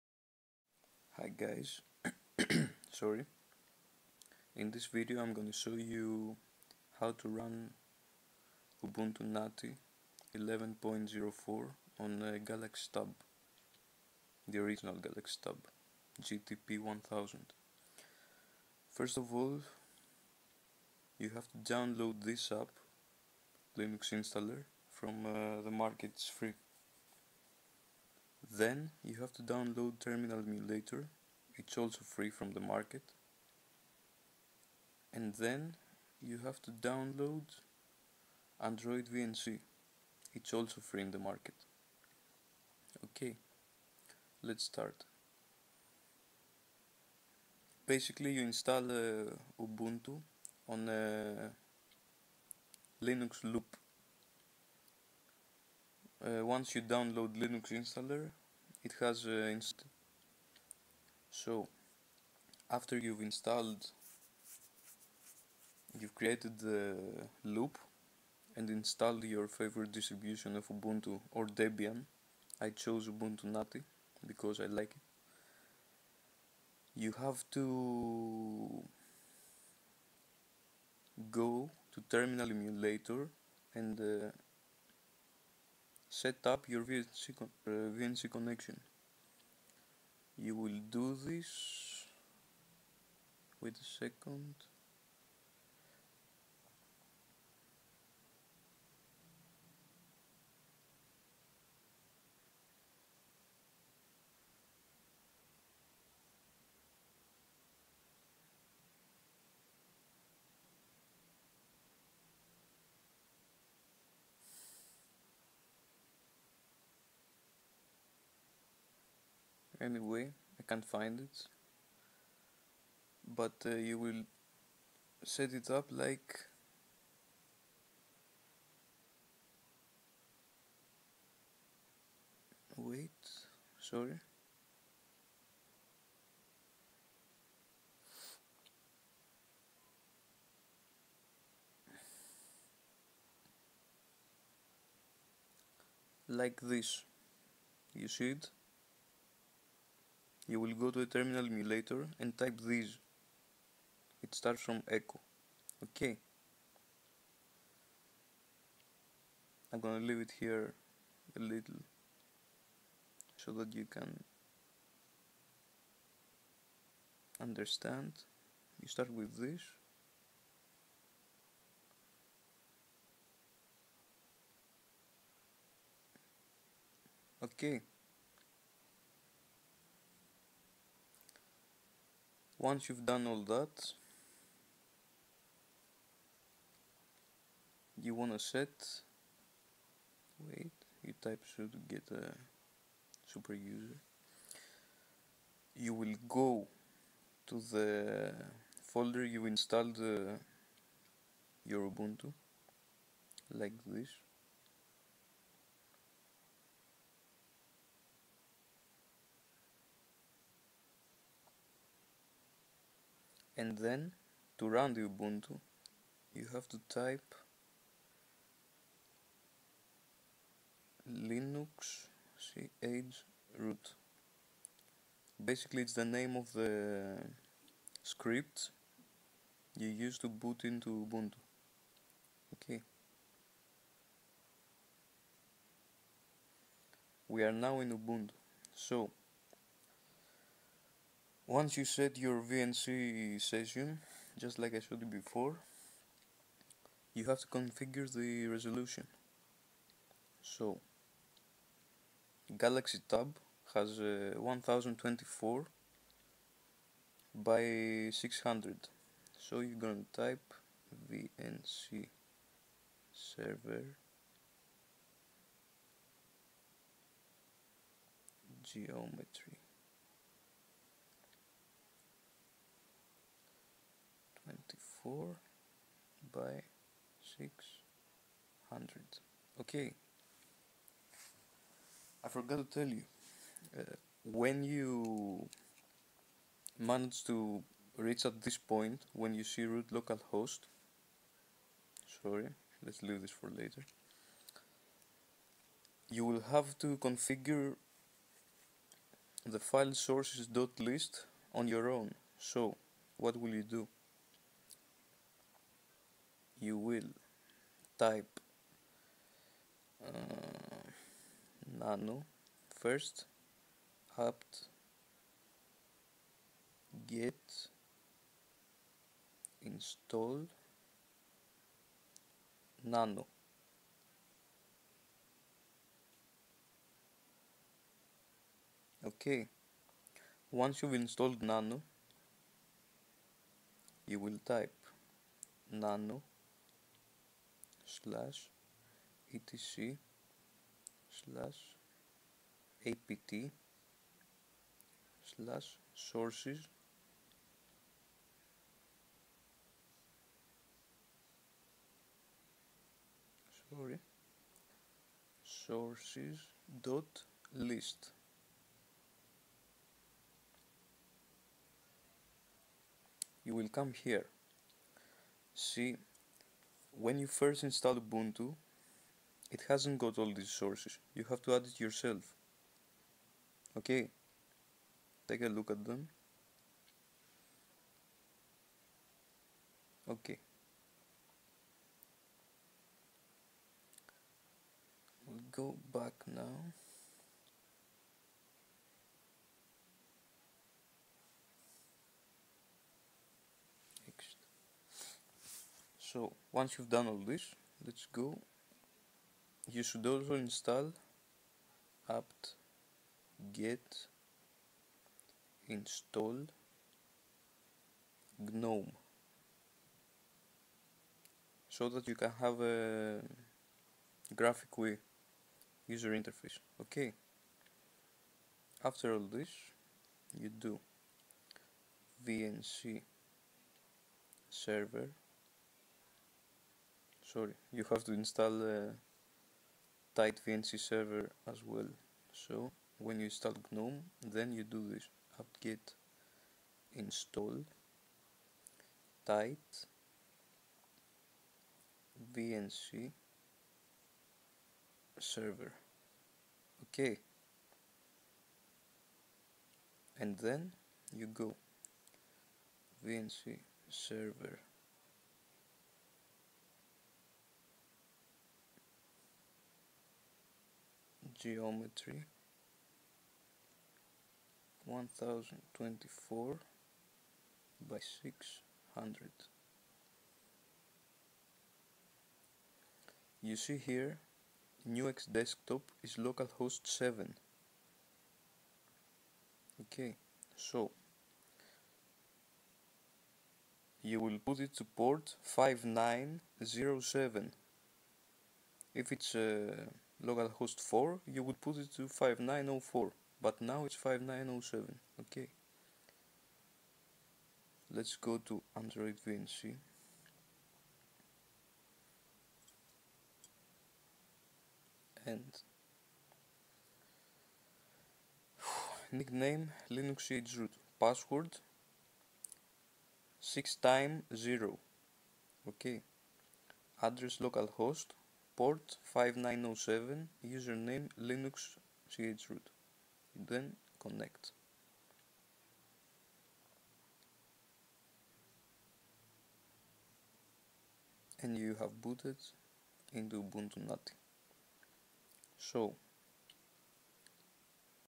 Ubuntu Natty 11.04 running on the Original Samsung Galaxy Tab 7'' Part 1
This is Ubuntu Natty 11.04 with flash support running on the Original Samsung Galaxy Tab 7''
You will need these applications (they are all free from the Market):

Linux Installer by Galoula
AndroidVNC
Terminal Emulator

Also your tab must be rooted. I use Official XXJQ3 rom with CF-Root Kernel XXJQ1 and my tab is the wifi and 3g european version GT-P1000